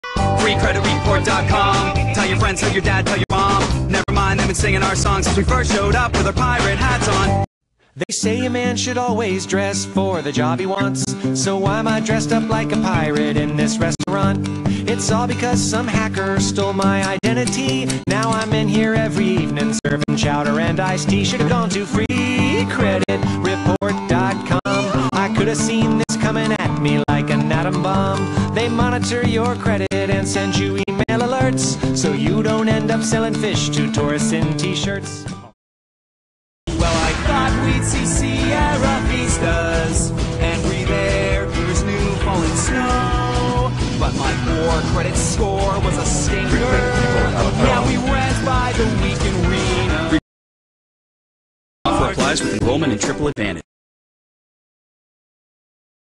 Freecreditreport.com old lady AKA Jacqueline Laforce
Update: the actress is Jacqueline Laforce, who plays "grandma" and she's not new to acting in commercials at least in Canada

The freecreditreport.com "band" aren't the only ones being replaced so is the grimacing old lady that appeared in many of the infamous commercials. Now we may never know her story. Was she the singer's mom, disappointed that he allowed his credit to fall into such shambles? Was she an identity theif who was angry about this fabulous "free" service. Or was she just a very old groupie?

If you're like me and want to know who the Freecreditreport.com old lady is contact Martin Agency, the company that created the commercials, and demand they cast her for the new Freecreditscore.com commercials!